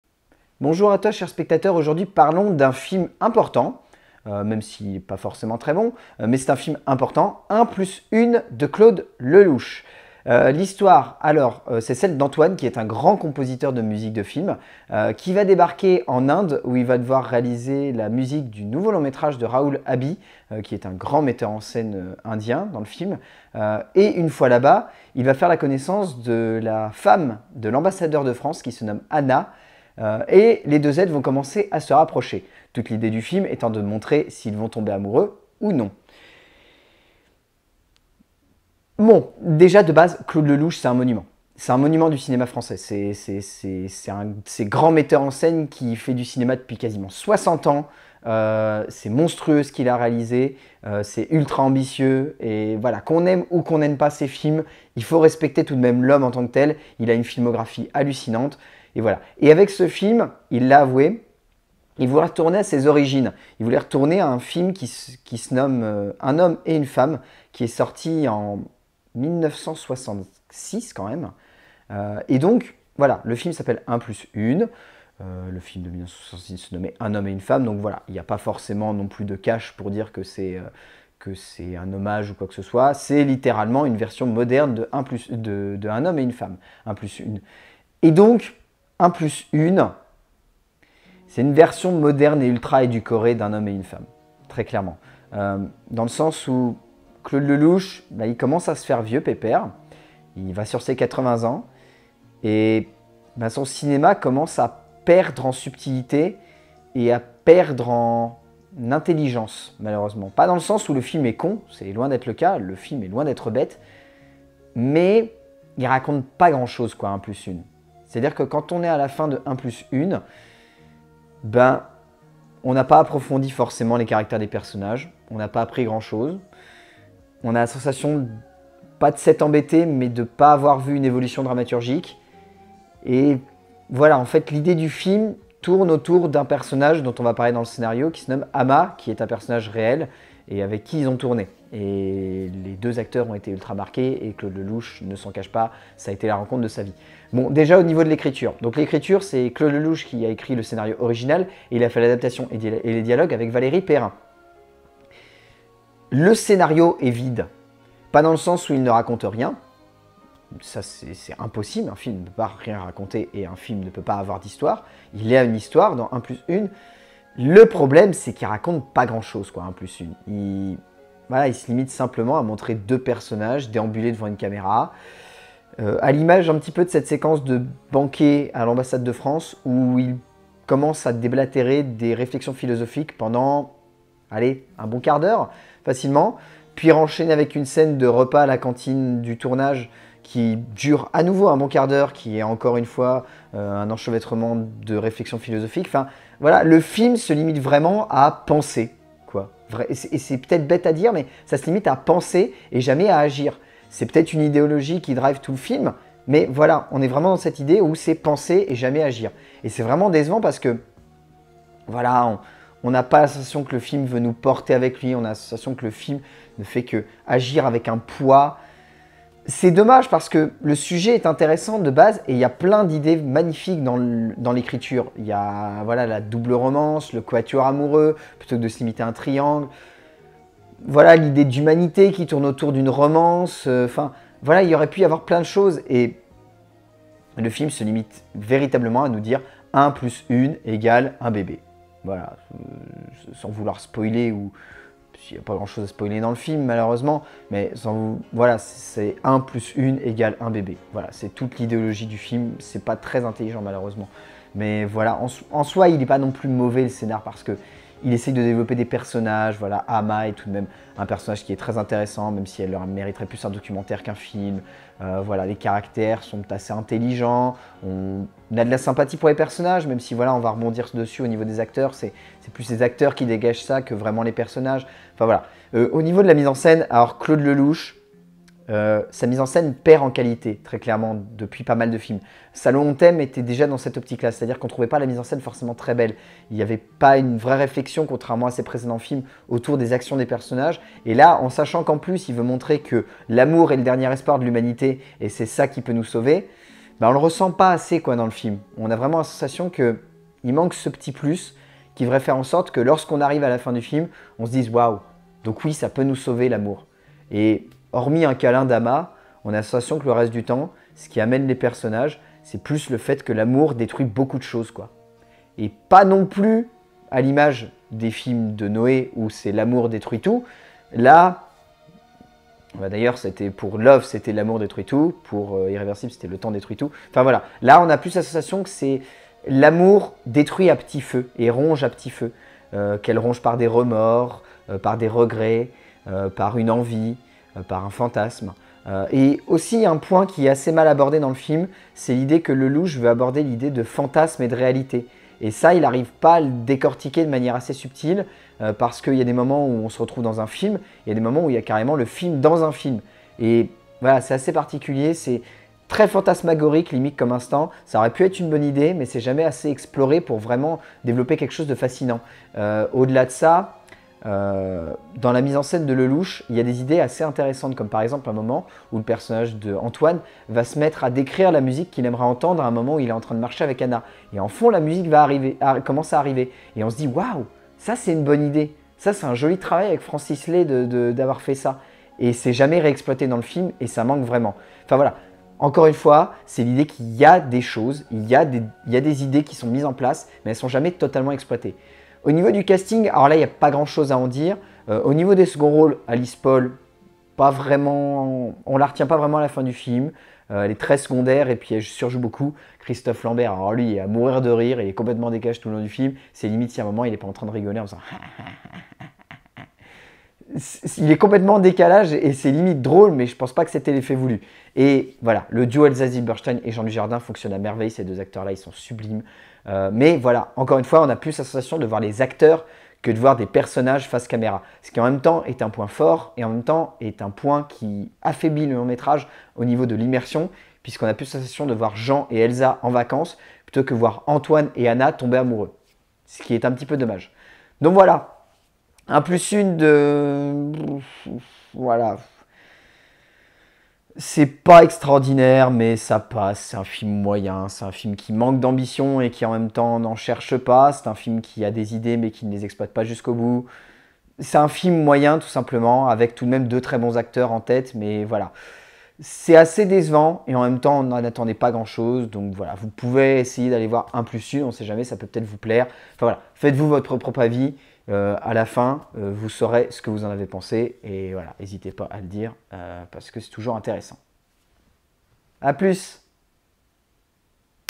LCDC - Un + Une
Vlog critique sur la sortie du dernier long métrage d'un des plus grands réalisateurs français encore en vie actuellement, un auteur rare, et même si il divise, assez important, "Un + Une"